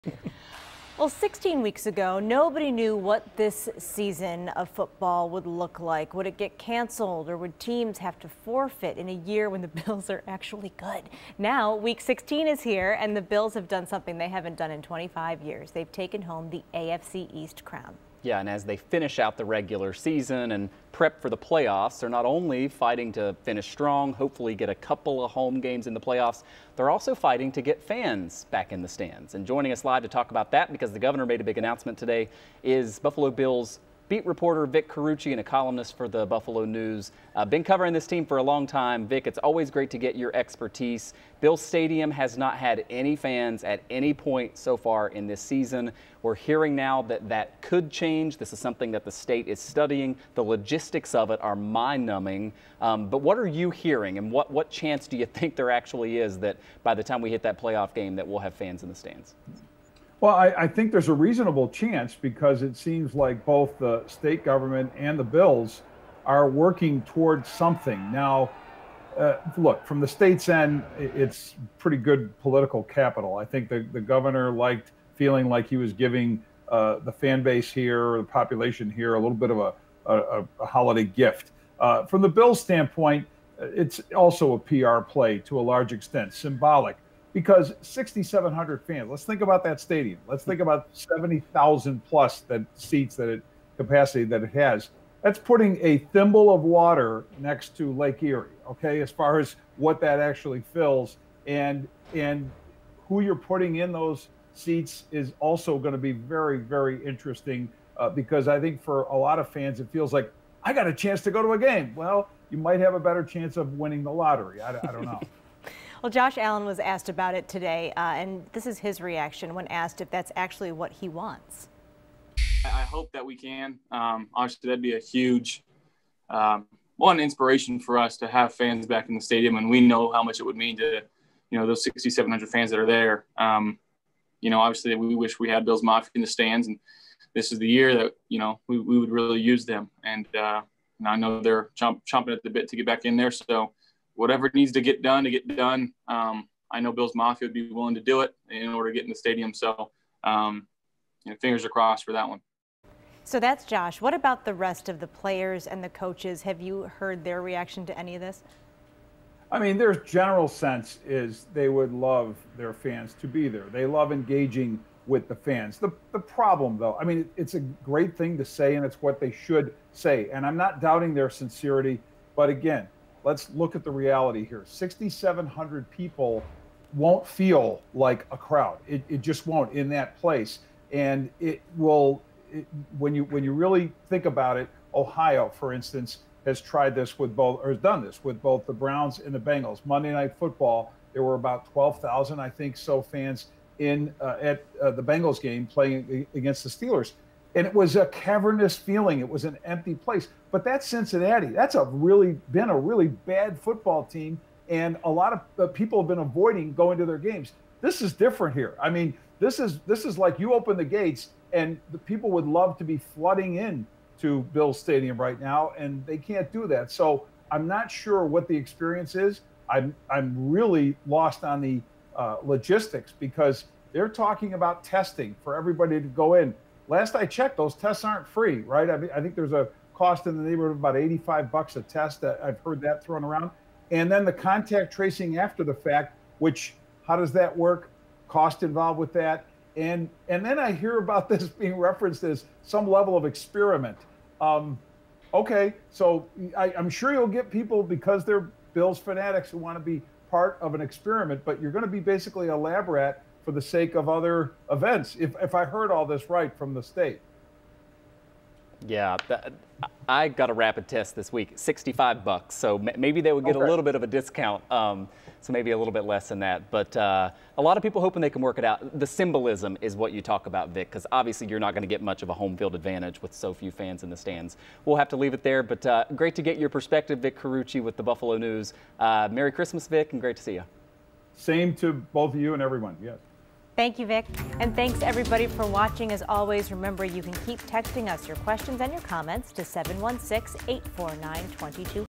Wednesday Town Hall Segment Three
Buffalo news Bills writer Vic Carucci joins our town hall to discuss possible fans in stand for playoff game